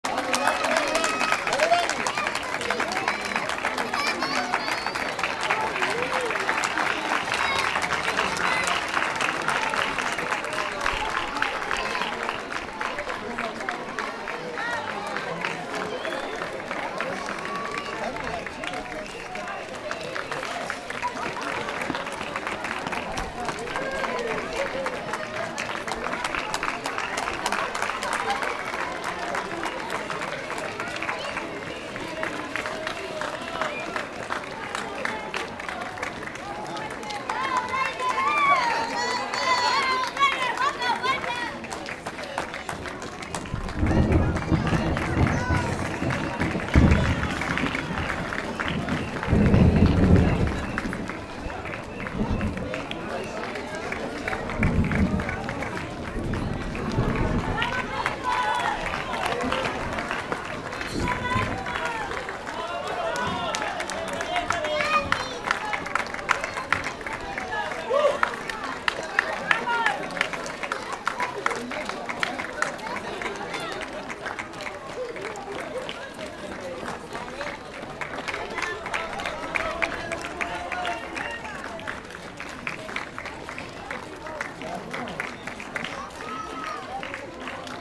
Llegada de otros corredores
Patzún, Llegada de otros corredores en la Carrera Independencia 56 Edición en la entrada a Patzún